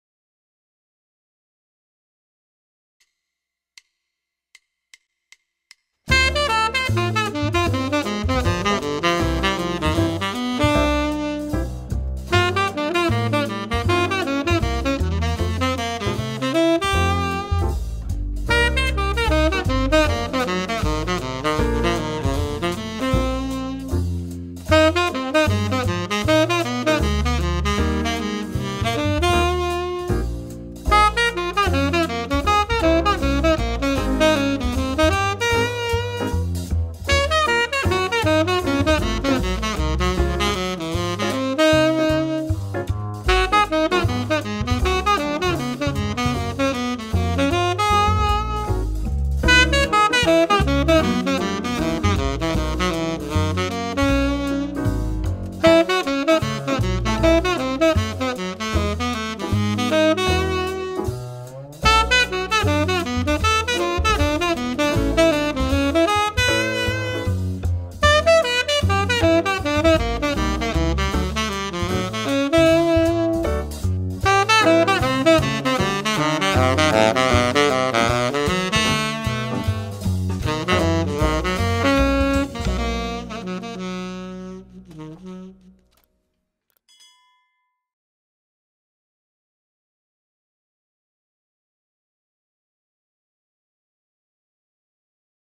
II-V-I pattern in 12 keys (Lick of The Week)
Here you are the 2-5-1 inspired on a Charlie Parker ‘s phrase which we analyzed in our last video. If you haven’t seen it yet you can do it so as to understand how is developed the main idea.

Sound card Focusrite Scarlette 2i2
microphone -ADK A51